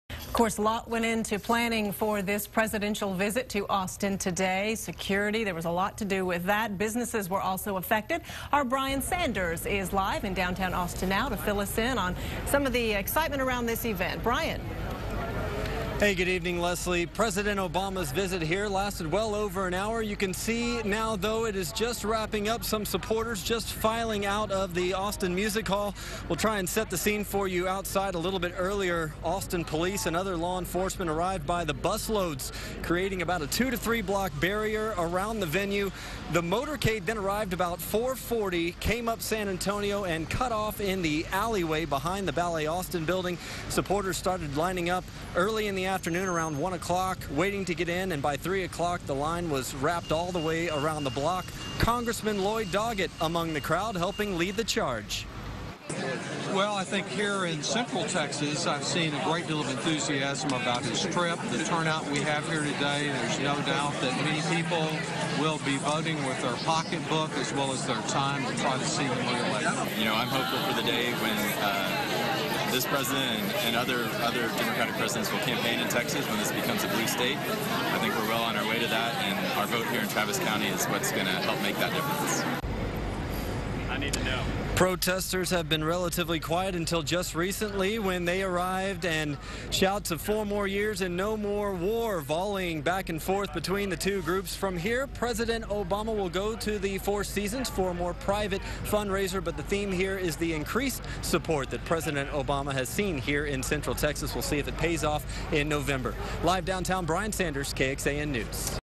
Preparations for president's arrival - 6 pm News
Sen. Lloyd Doggett commented on the president's visit to Austin. Protesters also had their say, each on opposite sides of the issues.